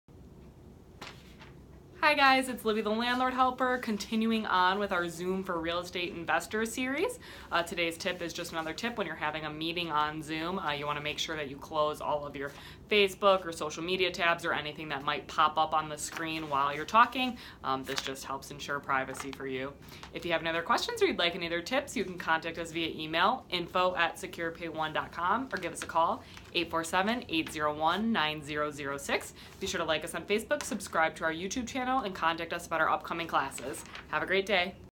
How To: Avoid Issues with Privacy on Zoom
NovemberSeries Zoom for real estate investors. Today's LibbyTip, be sure to close all social media, email, and messaging alerts when sharing your screen in a Zoom meeting to ensure your privacy!